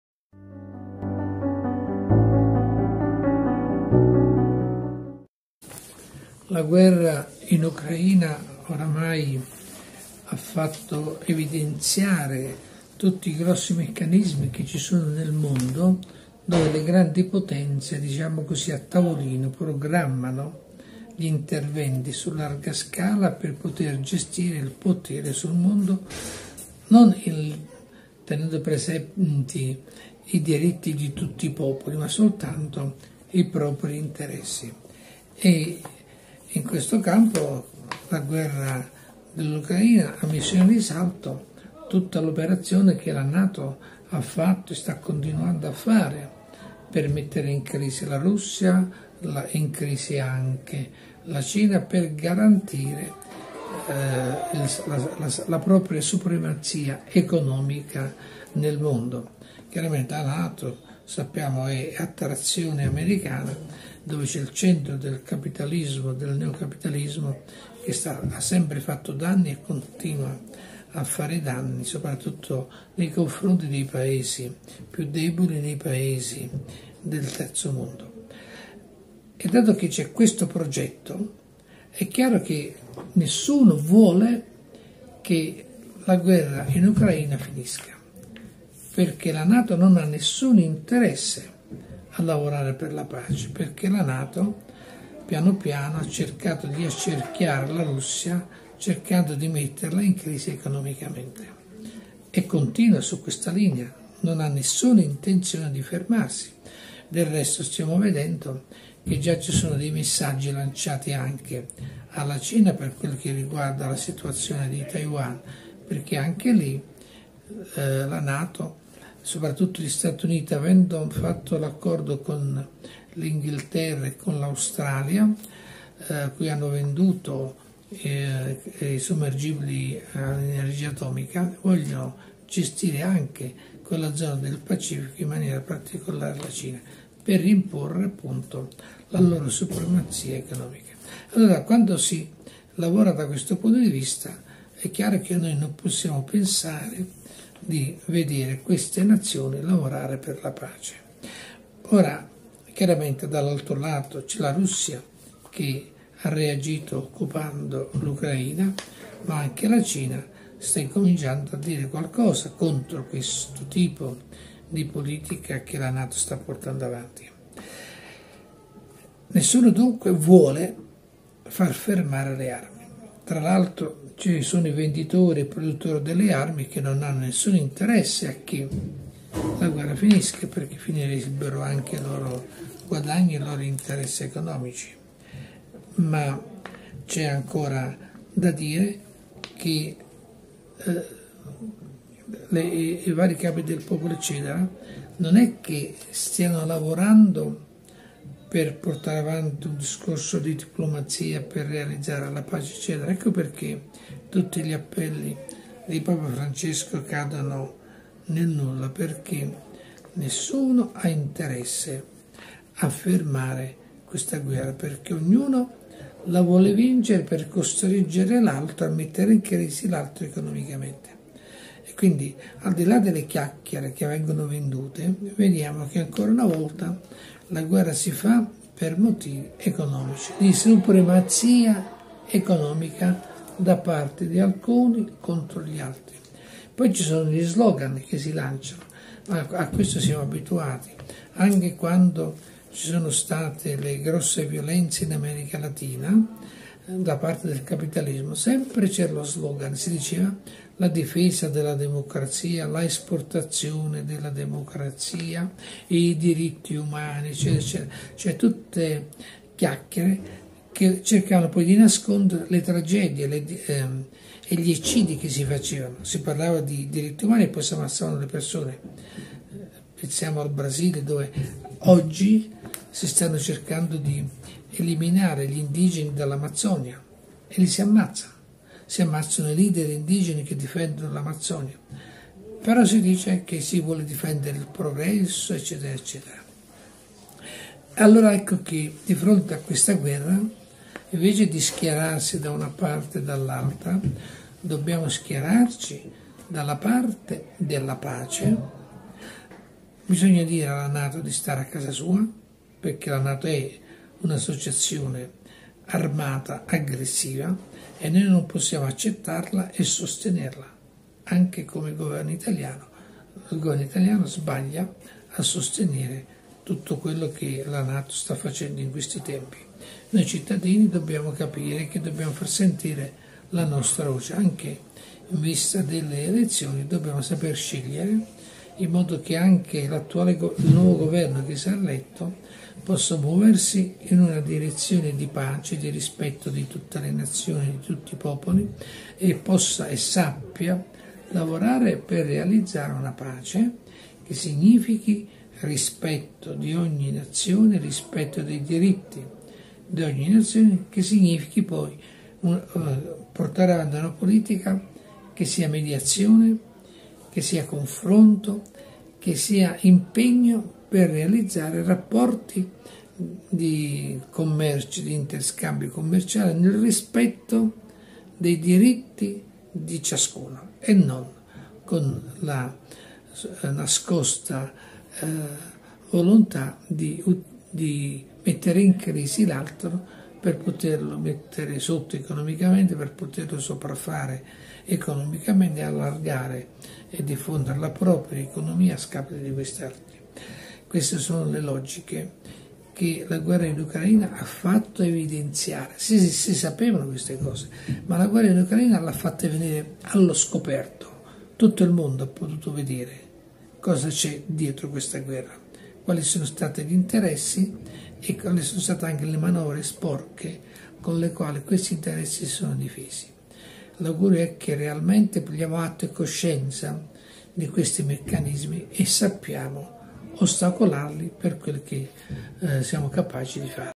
Guerra Russia-Ucraina
Riflessione di don Francesco Corbo della parrocchia dei santi Anna e Gioacchino.